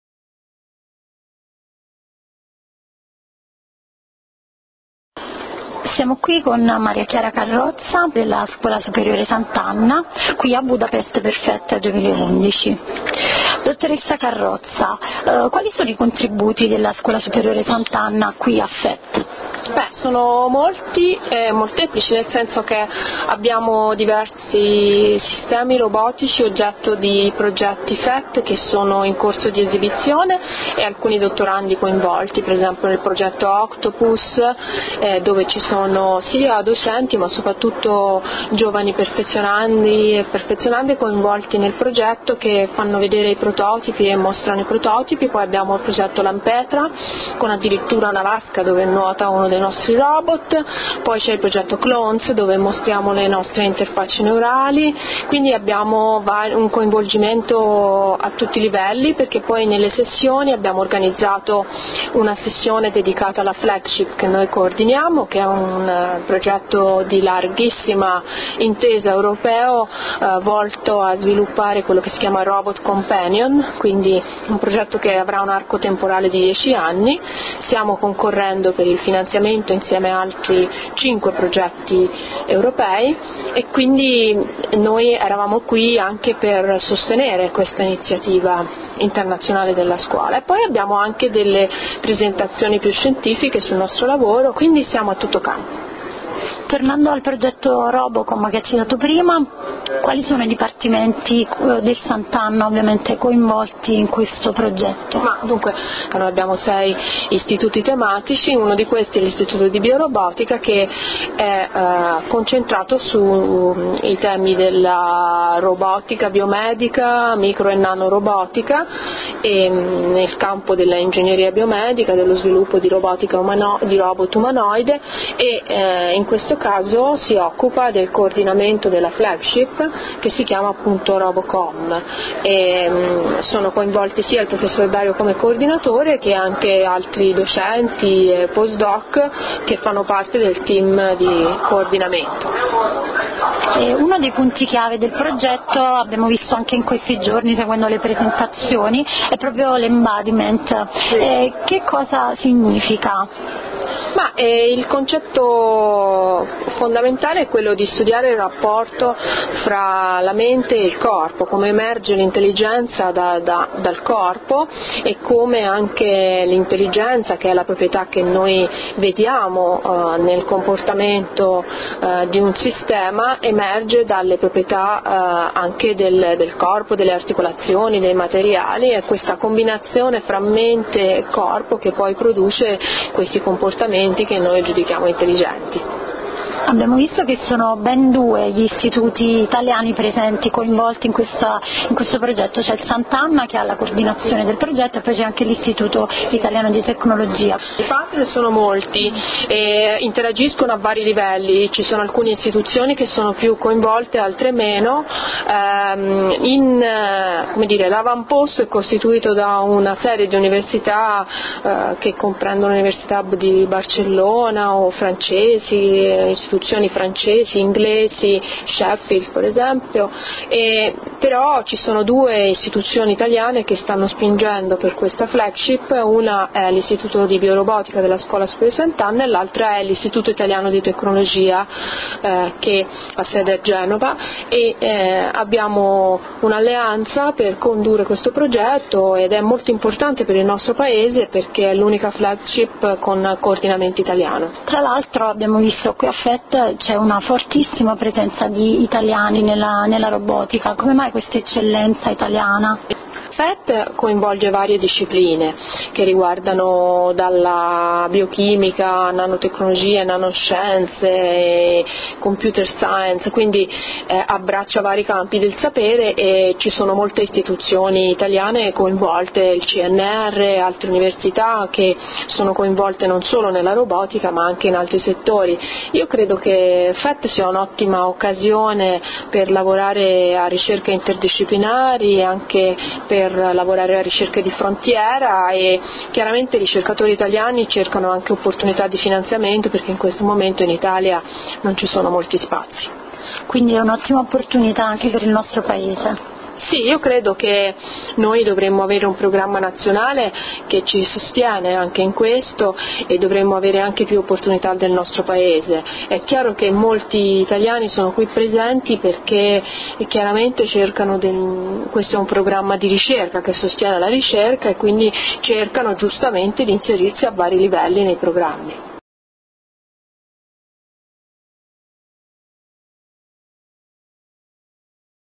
Intervista a Maria Chiara Carrozza al FET 2011
Intervista al direttore della Scuola Superiore Sant'Anna di Pisa Maria Chiara Carrozza sulla partecipazione della scuola alla conferenza europea sulle tecnologie emergenti FET 2011. Intervista di Marina D'Alessandro a Anna Davini, riprese e montaggio di Alessio Cimarelli. News